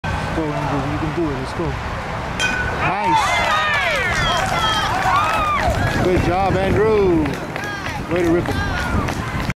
Andrew Santamaria Goes 6 For 6 Hit #6 An Infield Hit
Andrew Santamaria Goes 6 For 6 Hit #6 An Infield Hit With 6 runs scored and 1 double For The Cubs. He earned the game ball and the game was played on February 27, 2021.